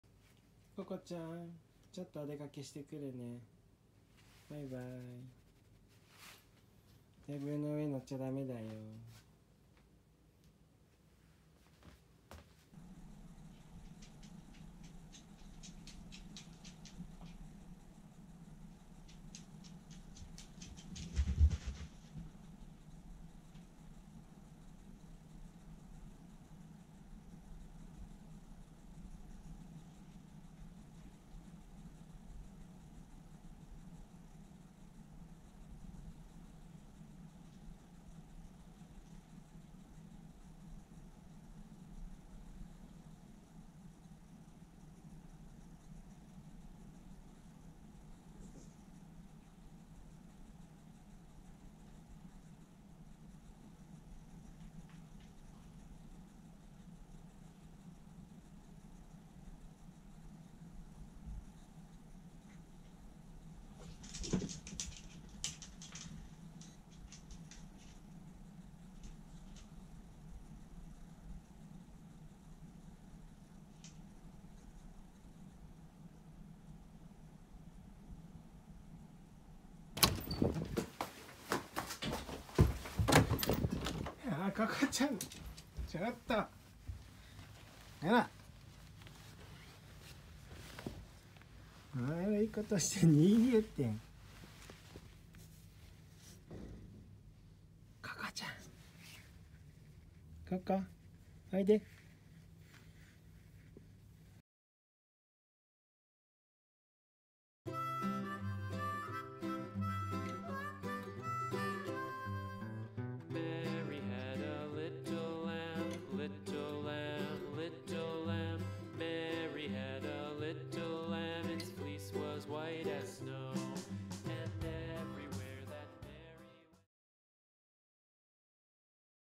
テーブルの上にのるいたずらっ子 ミニチュアダックス ココちゃん mini dachshund Guilty dog reprimanded
テーブルの上に乗りたがるココちゃん

Coco know self have done something wrong